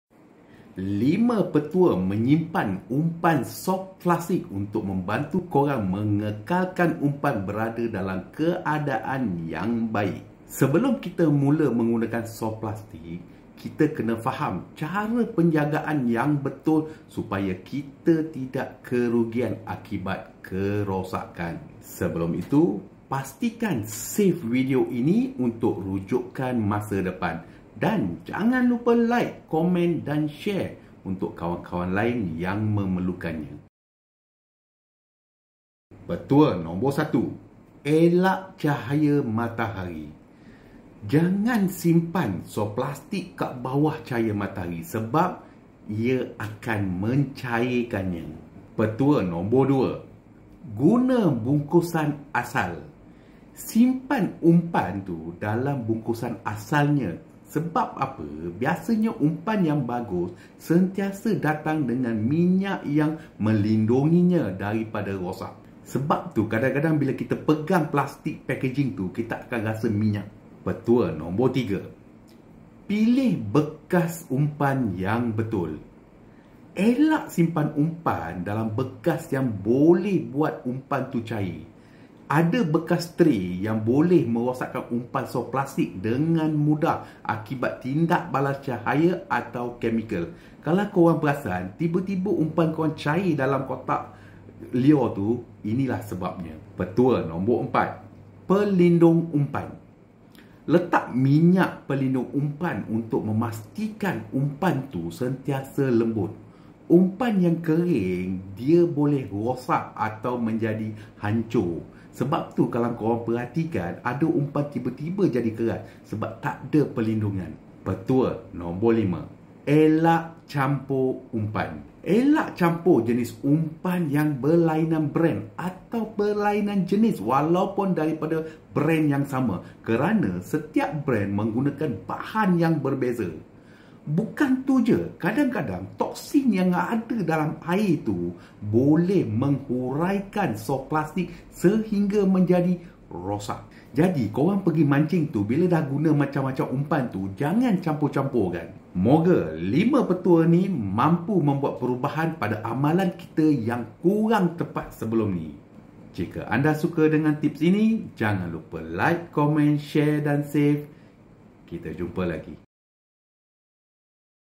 TIPS PANCING: 5 PETUA SIMPAN UMPAN SOFT PLASTIC
5 Petua Menyimpan Umpan Soft Plastic untuk membantu korang mengekalkan umpan berada dalam keadaan yang baik.

Sebelum kita mula menggunakan umpan soft plastic kita kena faham cara penjagaan yang betul supaya kita tidak kerugian akibat kerosakkan.

Sebelum tu pastikan tonton video ini untuk huraian yang lebih lengkap dan untuk rujukan masa depan.

1. Elak Cahaya Matahari
2. Guna Bungkusan Asal
3. Pilih Bekas Yang Betul
4. Pelindung Umpan
5. Elak Campur